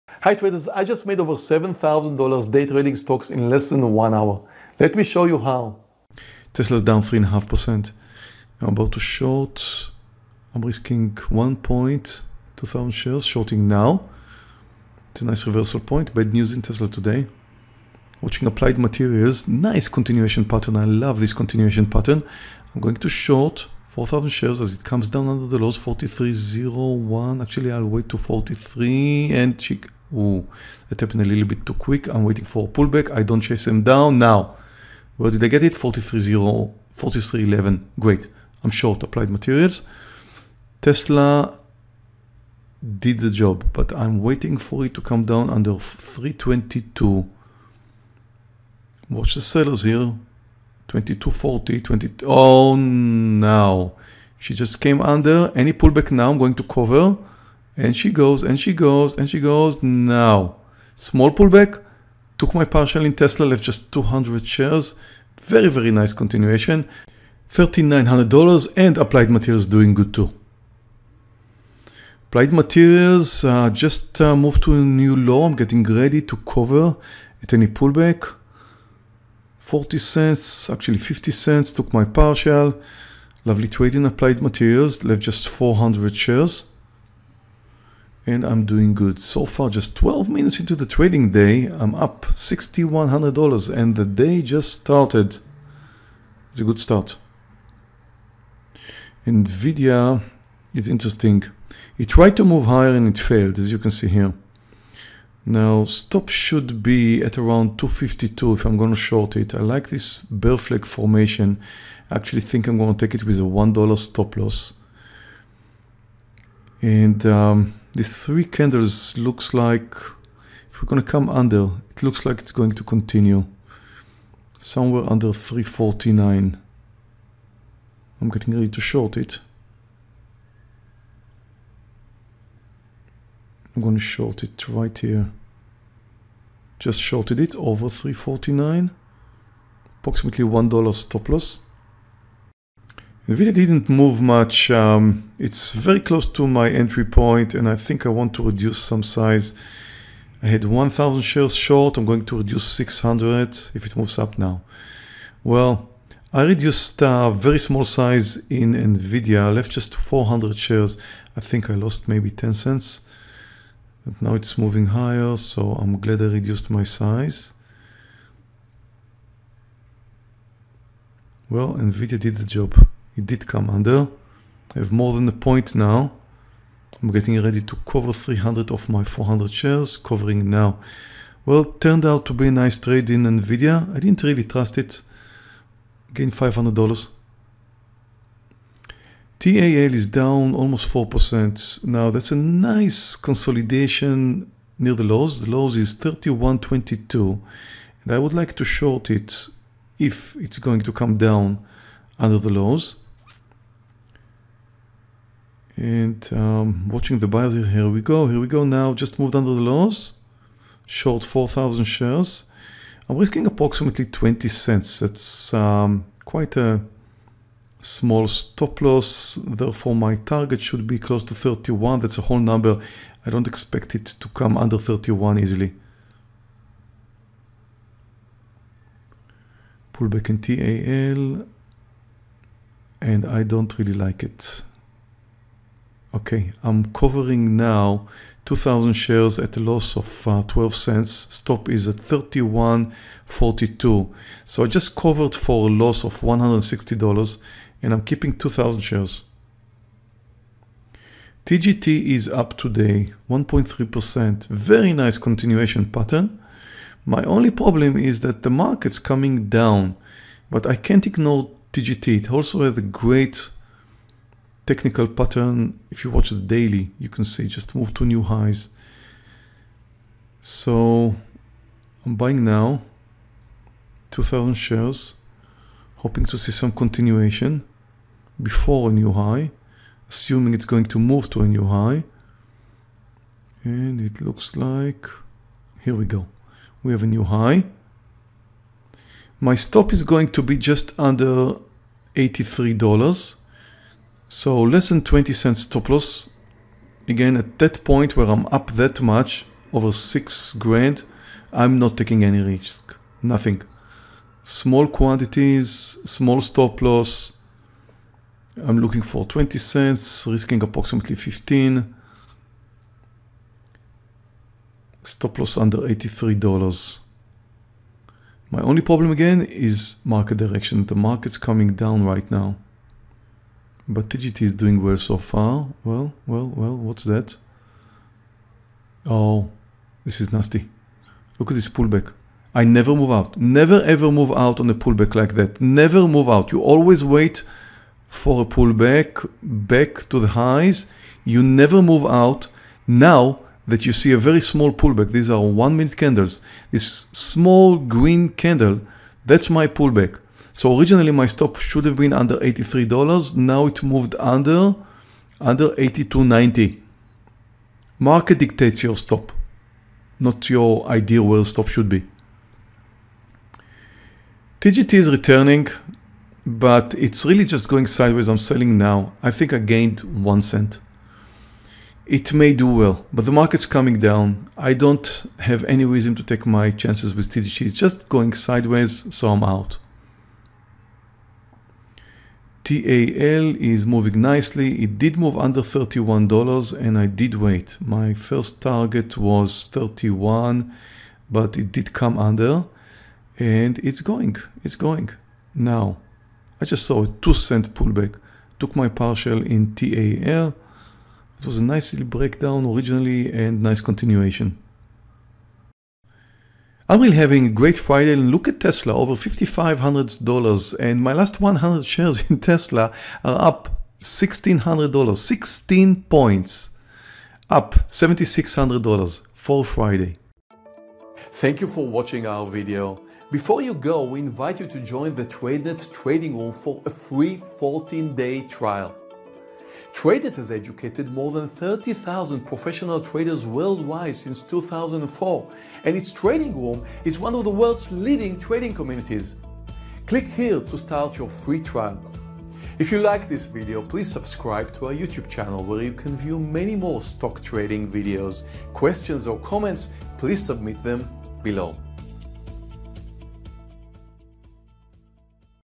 Day Trading Tesla for $7,600 Profit
Meir Barak day trading live on August 17th. 2018.